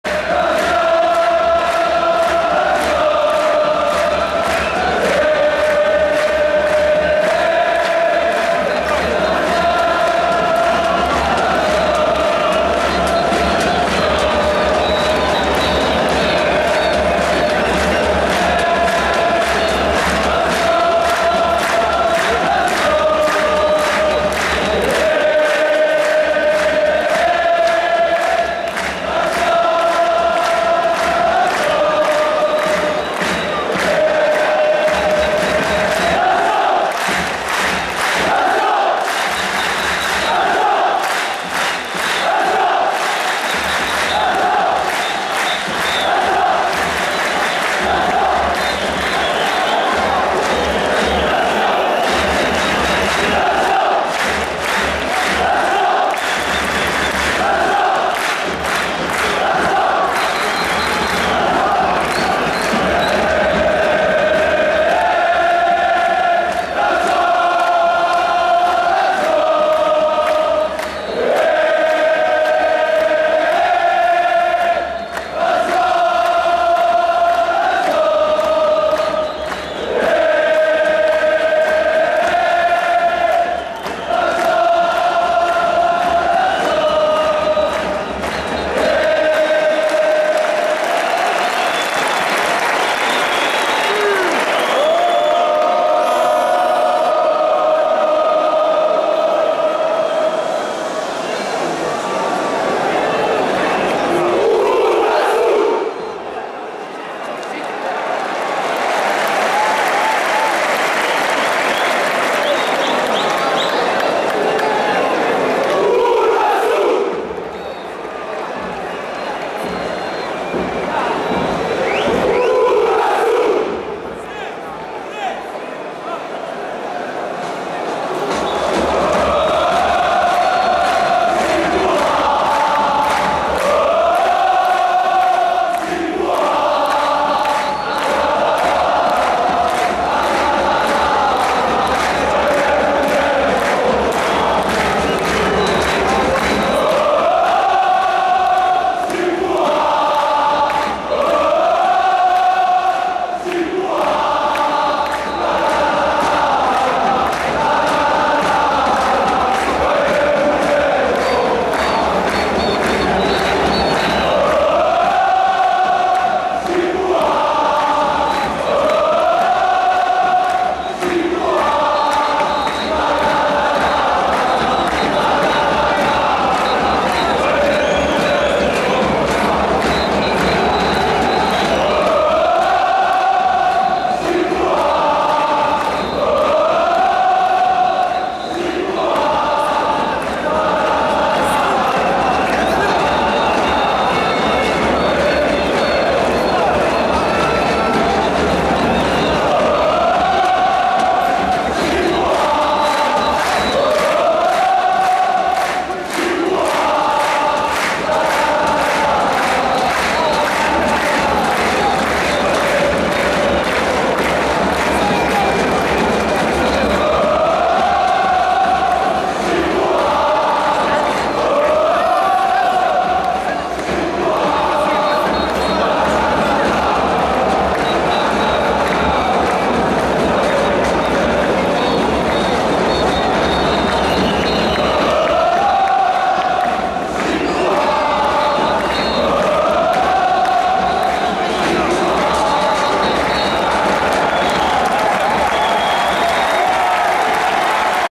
Raja vs Mat 0 - 0 , بطريقة رائعة إستقبل جمهور الرجاء اللاعبين في مباراة المغرب التطواني
Raja vs Mat  0 - 0 lors de la 27ème journée du championnat du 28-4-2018,  بطريقة رائعة إستقبل جمهور الرجاء اللاعبين في مباراة المغرب التطواني
Page Officielle :
Chaine Zemama35/youtube